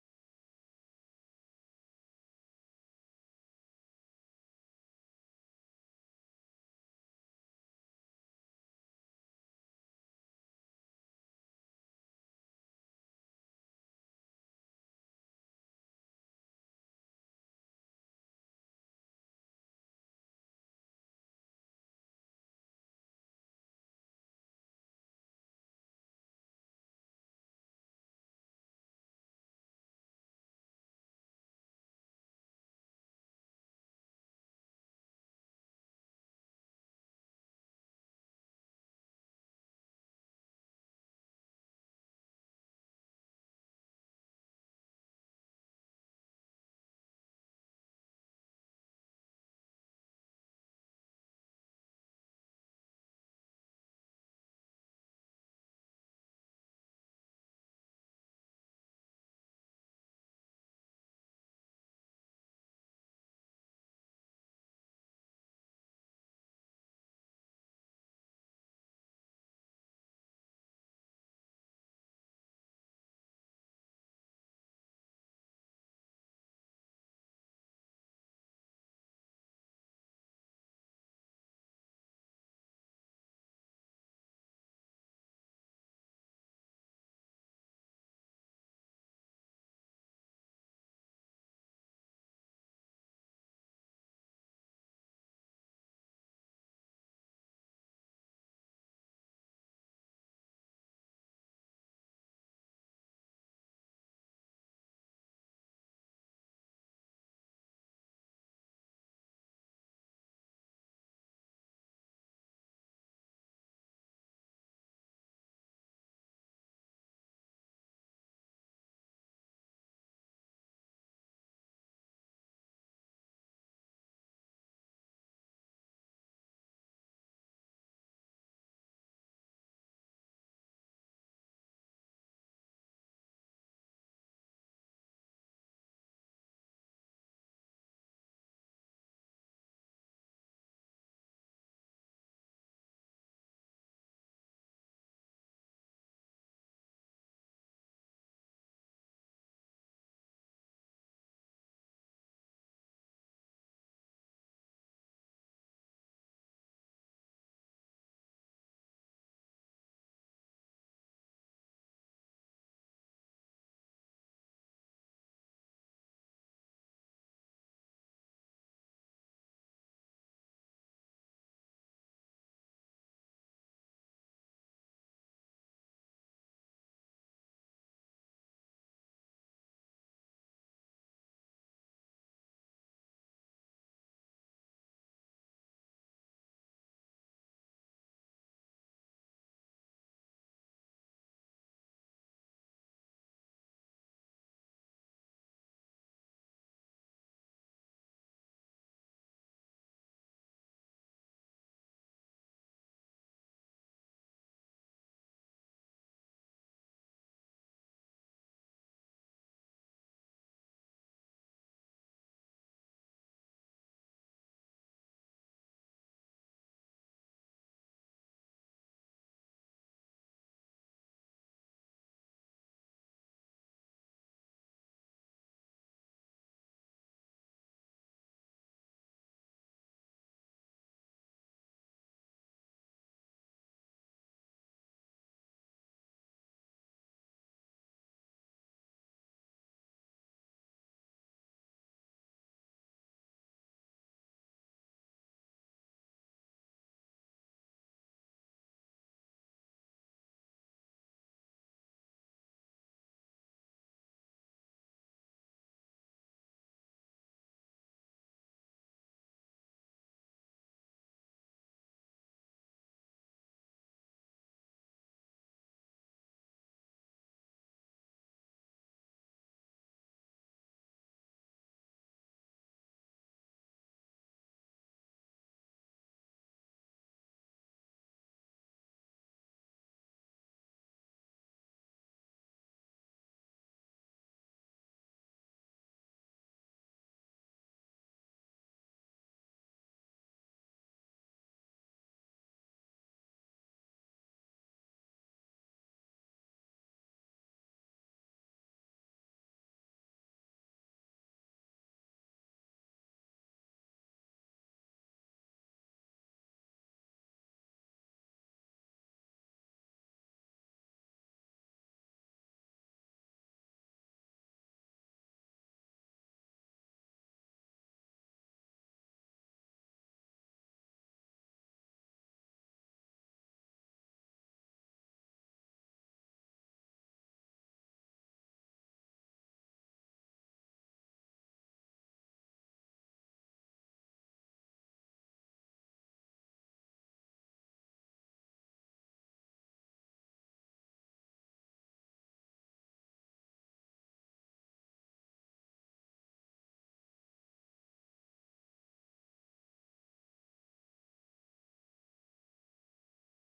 909 Checks Things Out at the Falls - July 1, 2023 (explore.org)
From explore.org's Brooks Falls Brown Bears cam and Brooks Falls Brown Bears Low cam at Katmai National Park